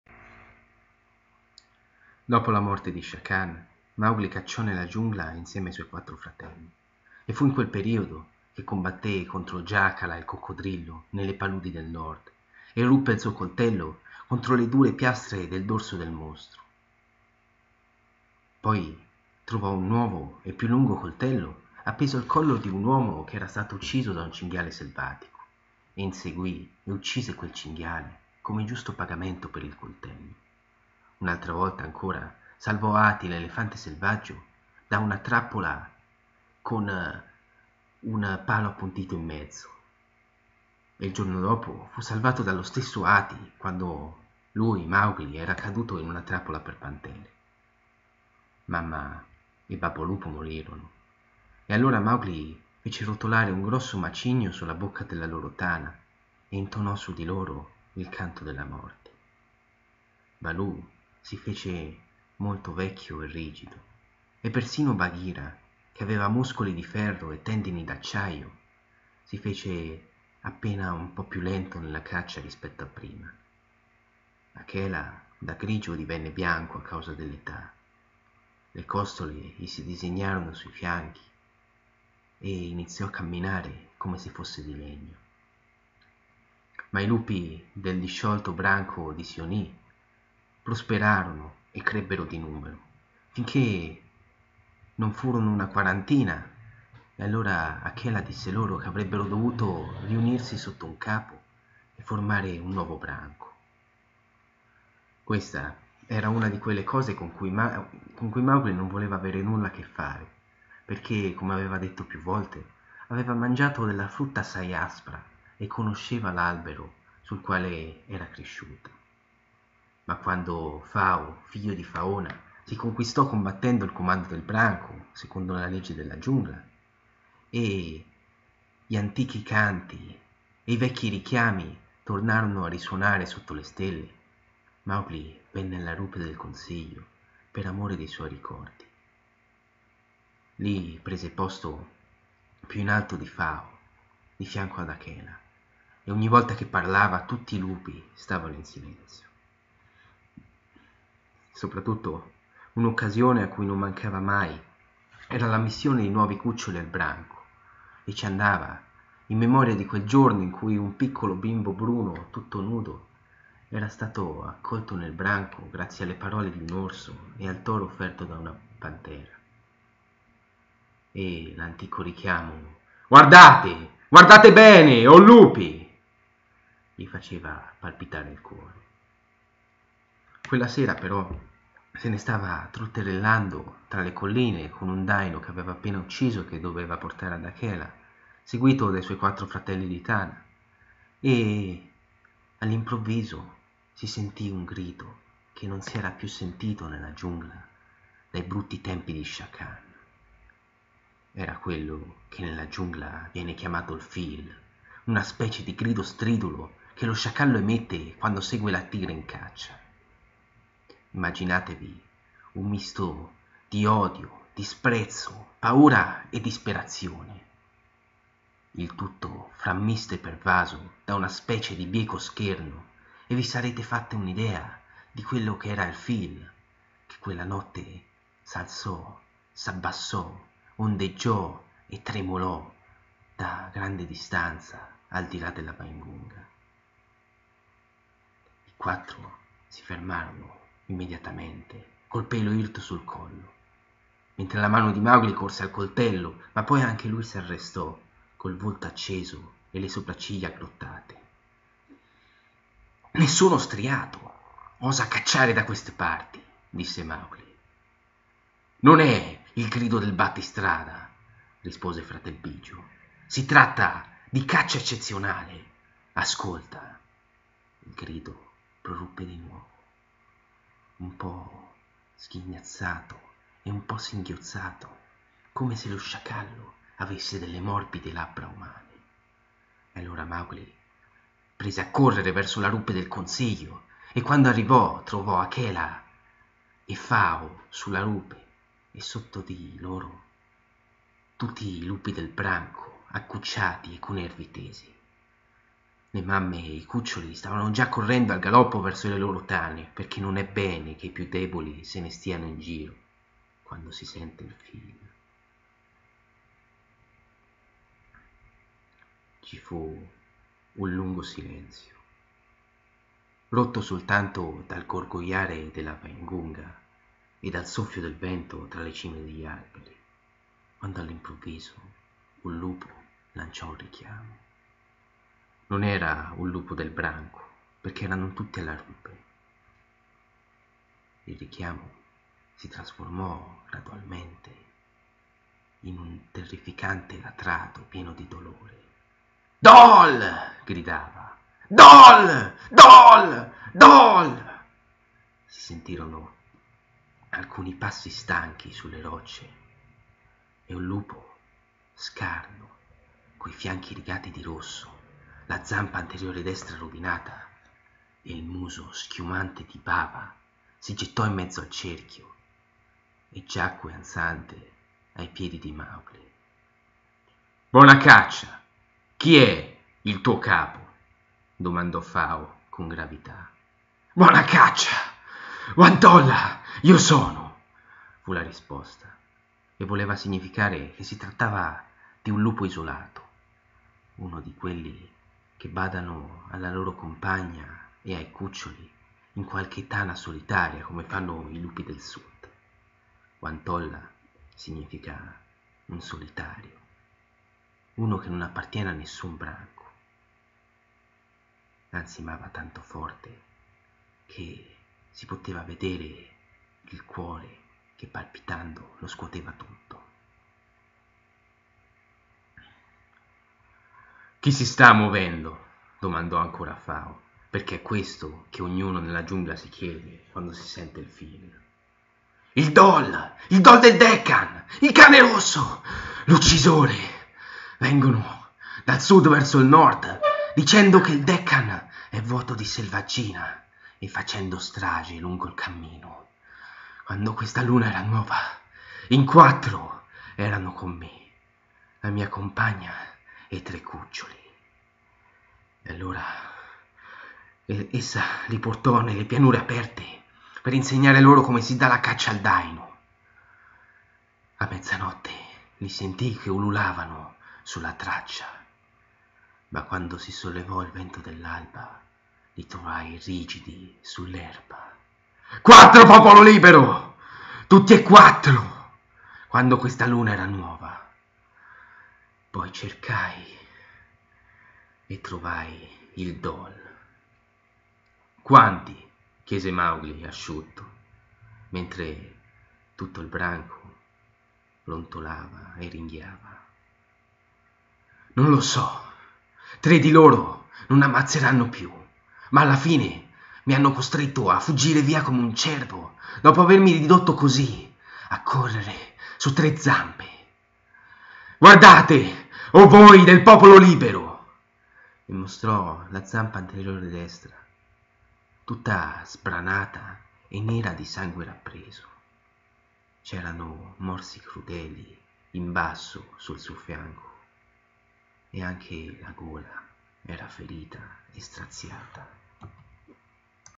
Cani Rossi 1 1 - Il libro della giungla
Il grido del Pheeal preannuncia una nuova minaccia per la Giungla e per il Popolo Libero. Won-Tolla, un lupo del sud, porta la notizia dell'arrivo dei dhole, i terribili cani rossi del Dekkan.
Paragrafi 1-13